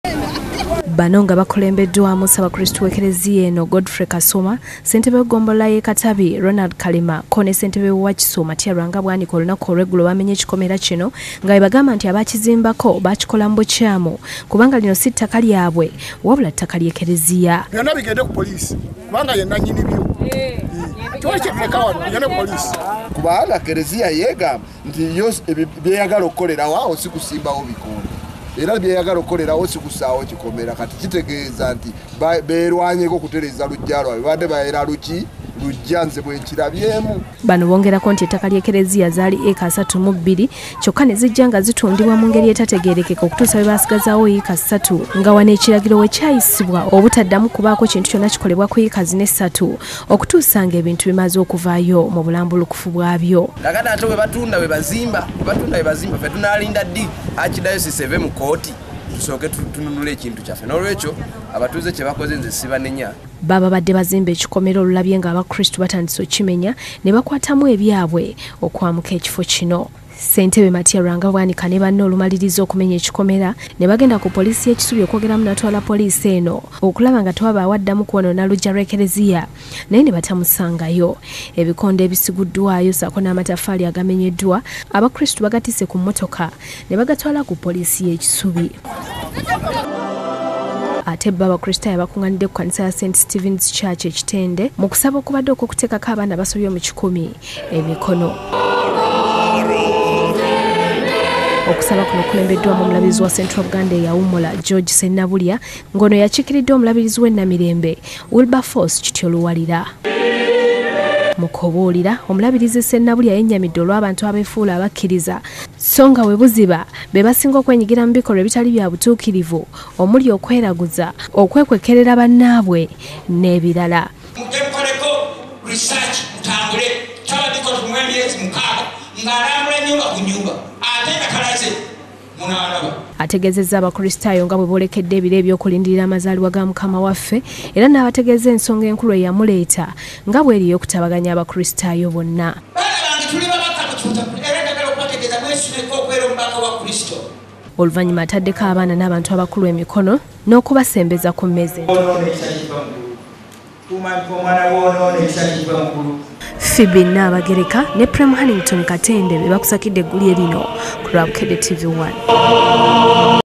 Abakristu bamenye ekikomera ky'Eklezia
Bakitadde ku nkyaayana za ttaka.